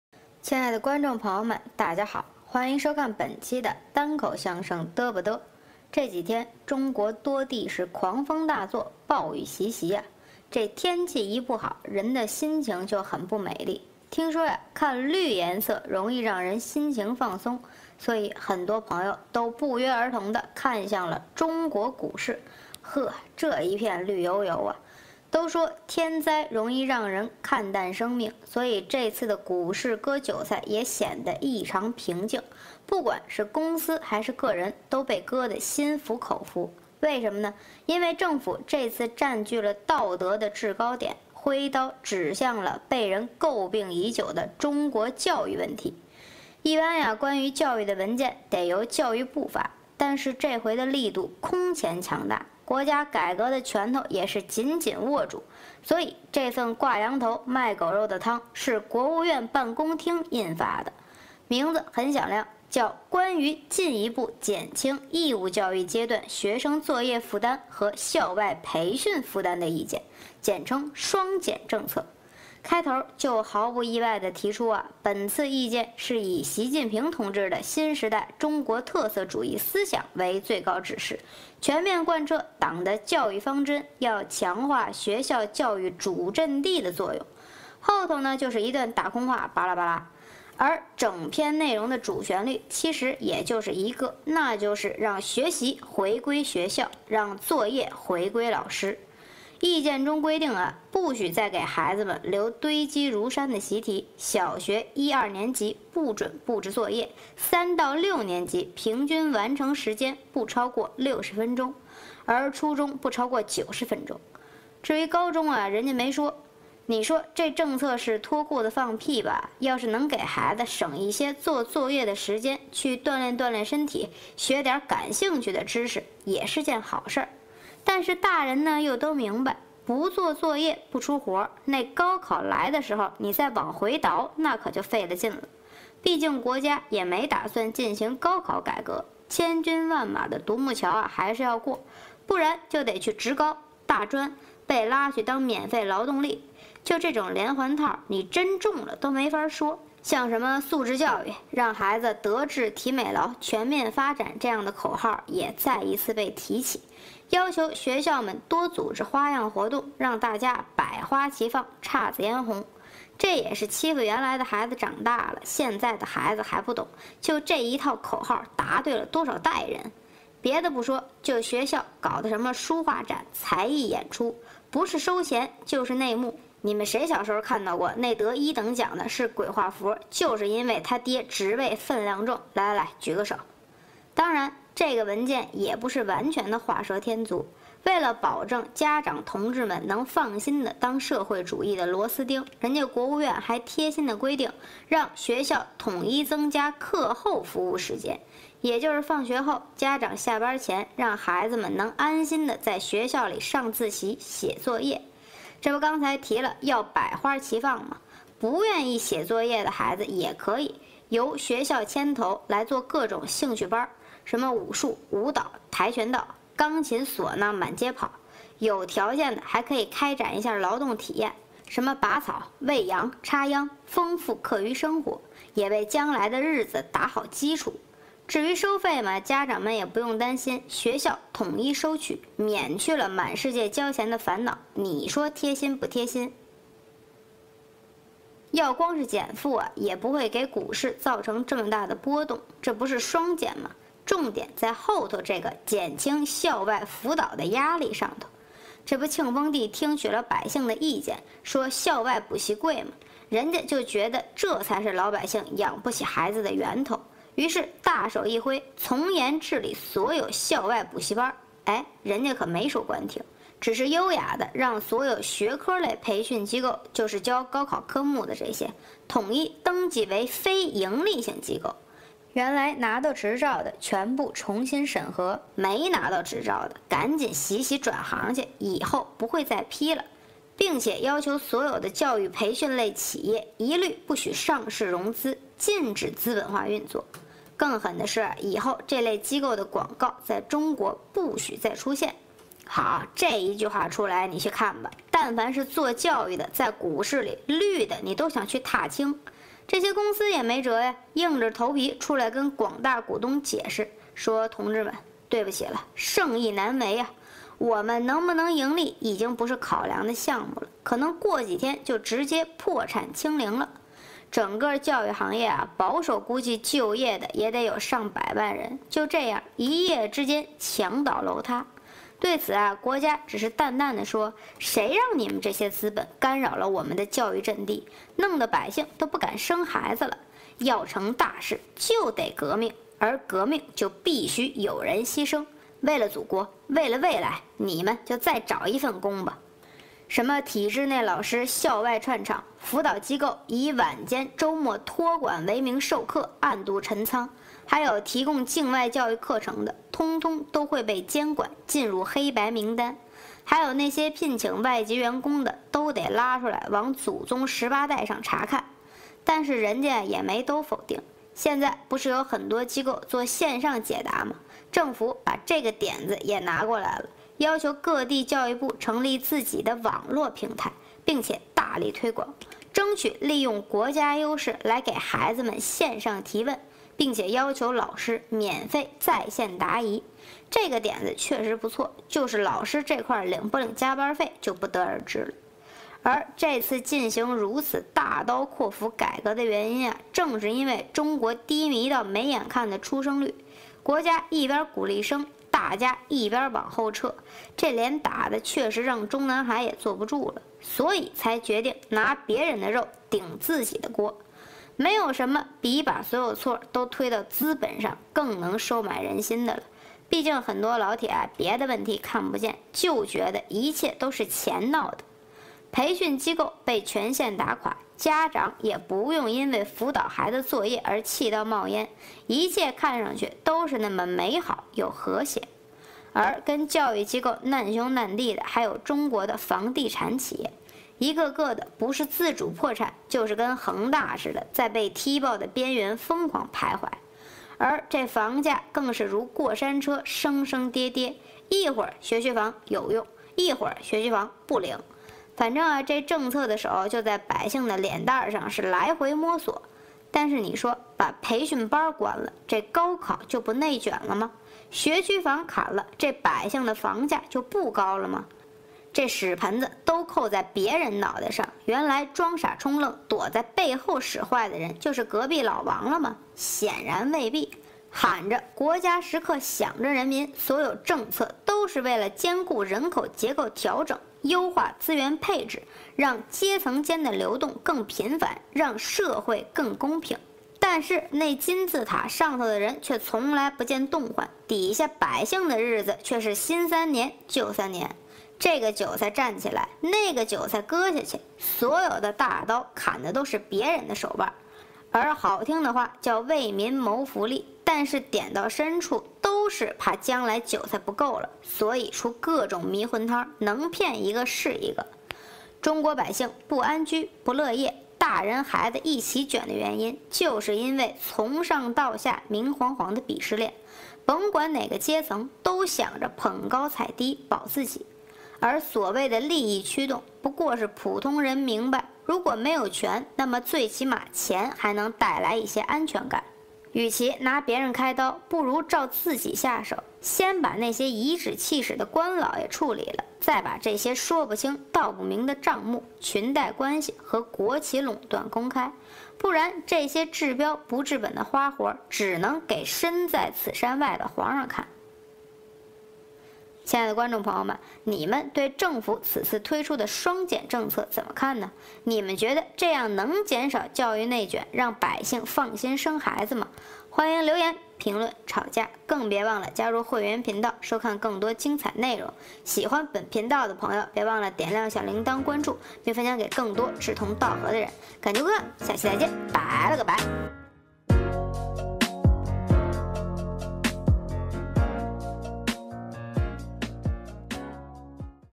北京上海迎来教育改革，快乐童年确保长大搬砖。中国双减政策发威，校外补习班被关停，导致教育股票大跌。不许留作业，提倡素质教育，然而高考内卷不变。是公平，还是为了催生韭菜玩障眼（单口相声嘚啵嘚之双减政策）
中国迎来强力教育改革，北京上海先行试点。双减政策要求学校成为教育主战场，杜绝孩子接触境外教材。并规定，作业少留，多锻炼身体和玩。然而高考内卷并没有变化，孩子们还是要在千军万马的战斗中争取考上大学。否则，进入大专后只有工厂和搬砖的明天。政府重拳打击校外补习班，不许宣传，并强制变成非营利机构，导致教育股全线崩盘。而学区房的起伏也让无数房地产企业走向崩溃的边缘。然而，对外下手改革真的能改变中国内卷的现状吗？充满焦虑感的百姓，究竟如何才能安居乐业。（单口相声嘚啵嘚之双减政策）